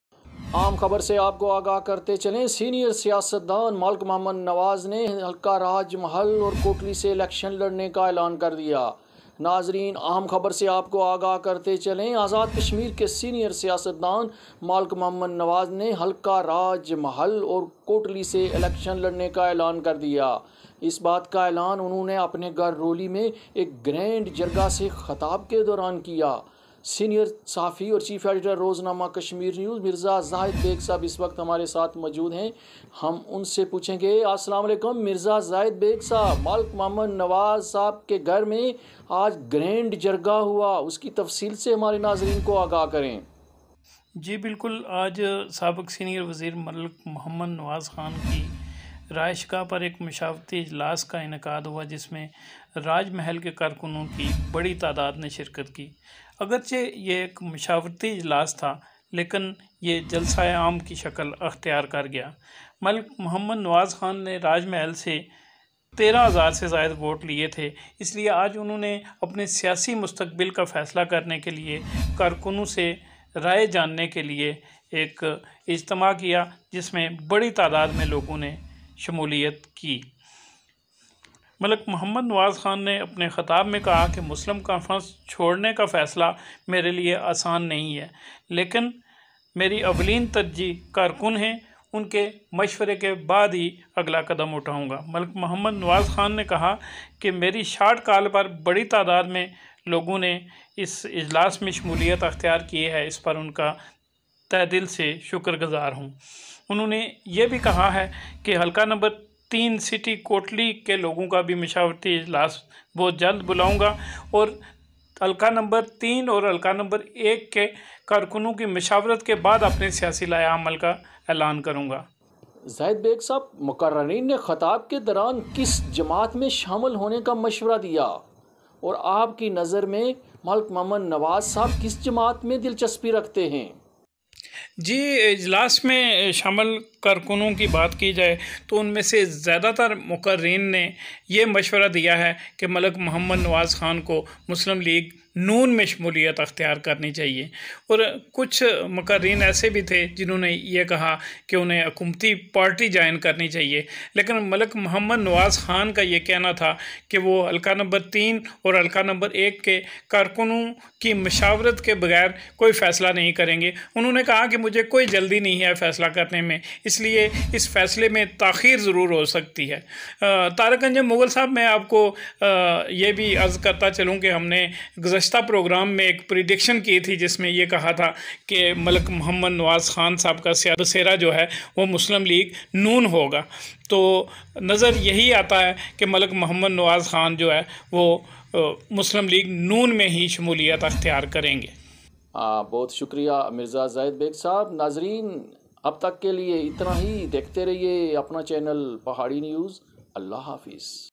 Malik M Nawaz Kon C Syasi Jamat Me Shamil Ho Rhy Khaber Samny Aw Gai | Kotli Azad Kashmir
Malik M Nawaz Kon C Syasi Jamat Me Shamil Ho Rhy Khaber Samny Aw Gai | Kotli Azad Kashmir | Pahari News AJK